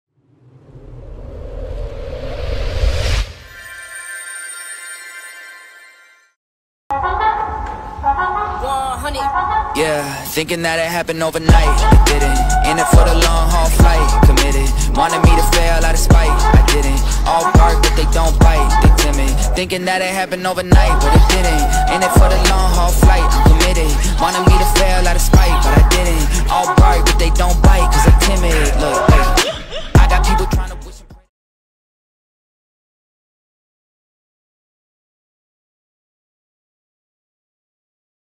Dimatur – Hexis – Hummer EV decorado Deep Black Satin
Novo Hummer EV decorado com o vinil Hexis SKINTAC HX20NPRS Deep Black Satin.😎🖤